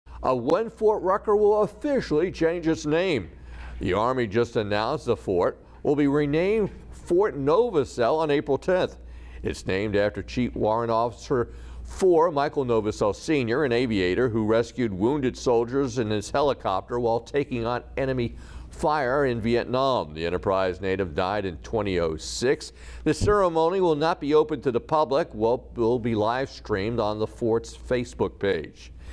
Official date set for Fort Rucker name change
On April 10, Fort Rucker will officially be changed to Fort Novosel during a ceremony held at Veterans Park on the post.